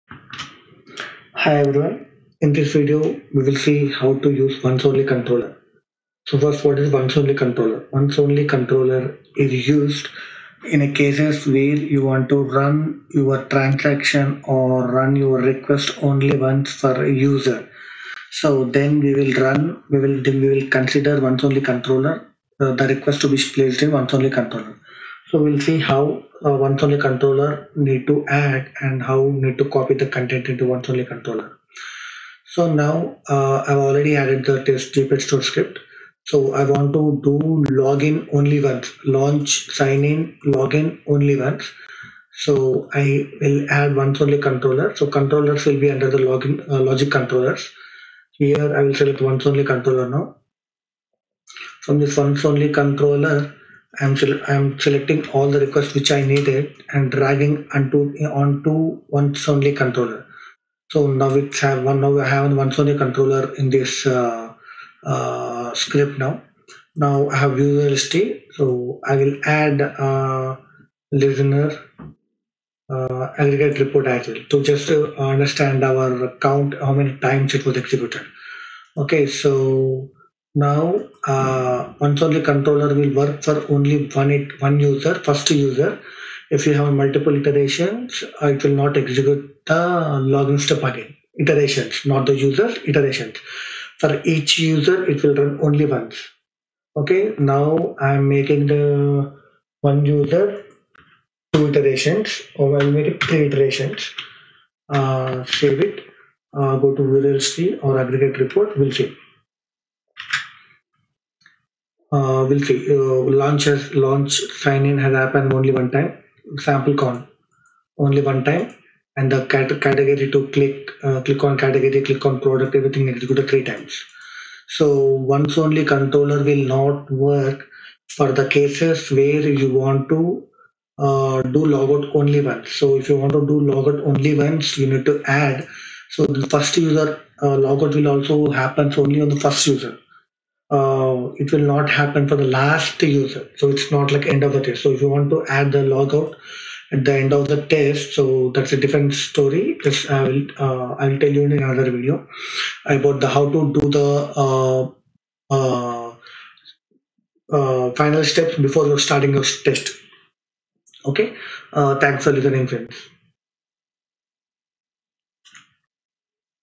How to Use Once Only Controller in JMeter | Advance Jmeter Scripting | Advanced JMeter Tutorial
How to Use Once Only Controller in JMeter | Advanced JMeter Tutorial

As a part of the JMeter Tutorial Series, In this video, we have discussed How to Use a Once Only Controller in JMeter.

What is the Once Only Controller?
As per Once Only Controller documentation: The Once Only Logic Controller tells JMeter to process the controller(s) inside it only once per Thread, and pass over any requests under it during further iterations through the test plan.